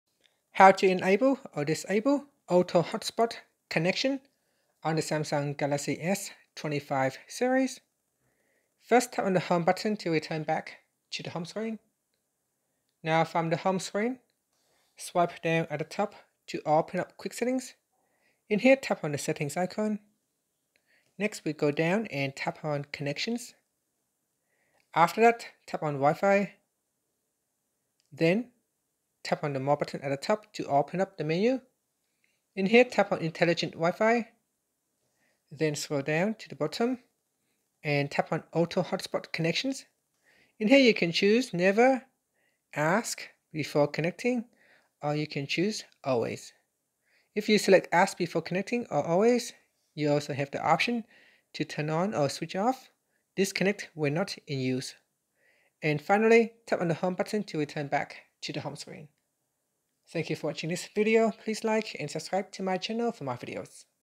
📶 How to Enable/Disable 🔥 Auto Hotspot Connection on Samsung Galaxy S25/S25+/Ultra
Learn how to enable or disable the Auto Hotspot Connection on your Samsung Galaxy S25, S25+, and S25 Ultra to seamlessly share internet with trusted devices. This feature lets your phone automatically create a Wi-Fi hotspot for nearby Samsung devices signed into the same Samsung account or connected via Family Sharing. Whether you want to conserve battery, control data usage, or ensure a stable internet connection, follow this quick and easy guide to turn Auto Hotspot on or off in your settings. Watch now and optimize your wireless connectivity!